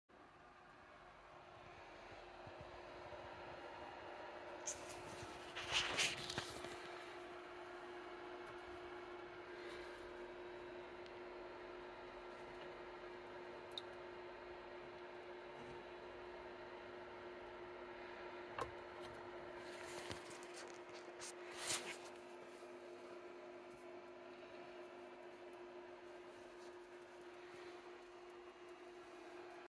Electrolysis cell gold bearing anode slimes
A sneak peak at the electrolytic cell i use to recover gold bearing anode slimes before treating chemically and refining.